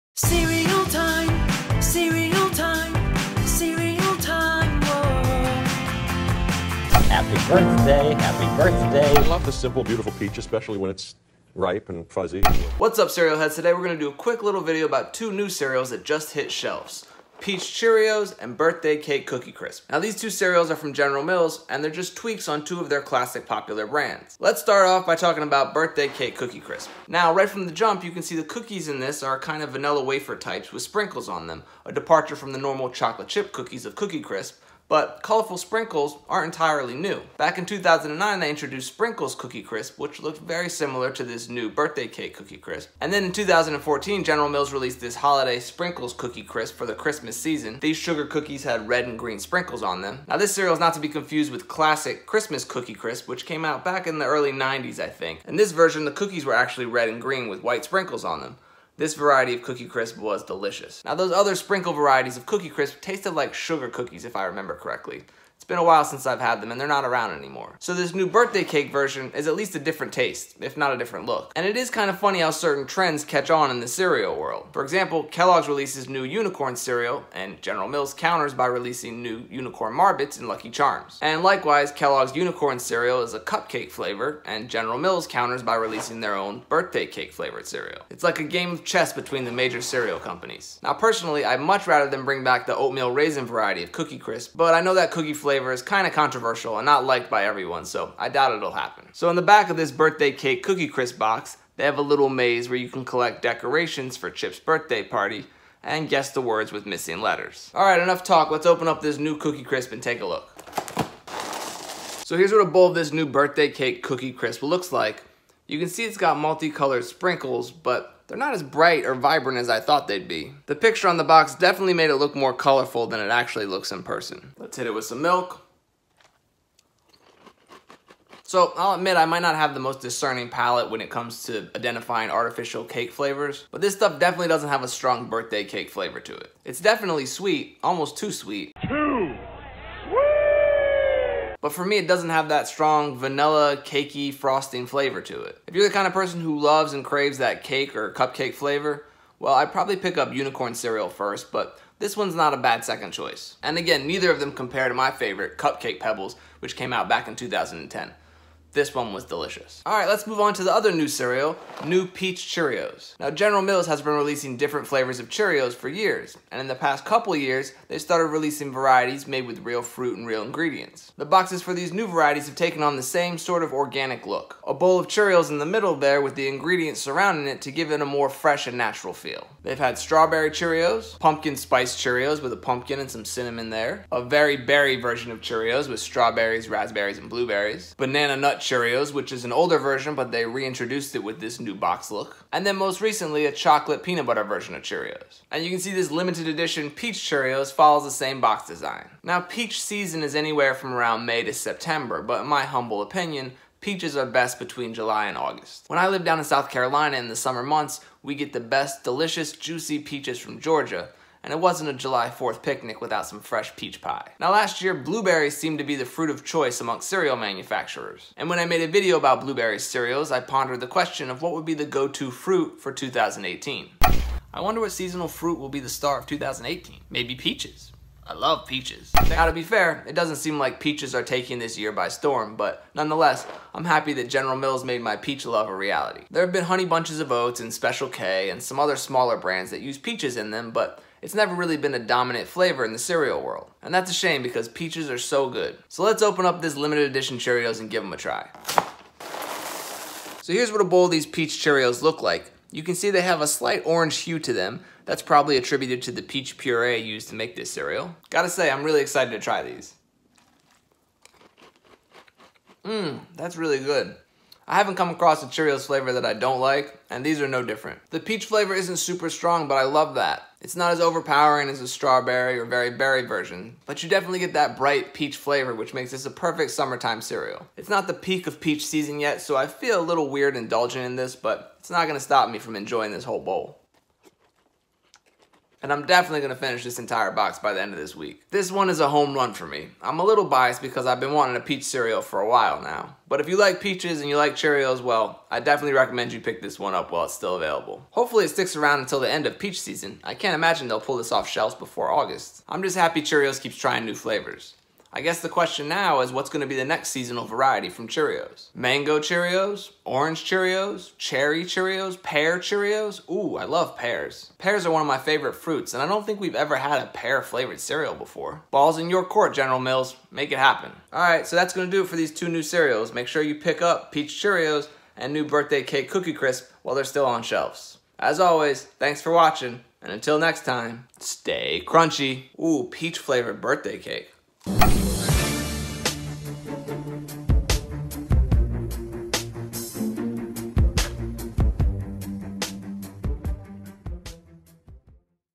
Peach Cheerios (2018) & Birthday Cake Cookie Crisp (2018)
General Mills just hit us with two new cereals -- small tweaks on two of their classic, popular brands. I'm so happy to finally have a Peach flavored Cheerios and I'm excited to see what other flavors they add to the Cheerios line in the future. The Birthday Cake Cookie Crisp is forgettable, but I can't complain about getting new cereal boxes!

You can find me (and Cereal Time) on social media at...

And make sure you check out CEREALOUSLY for cereal reviews and cereal news!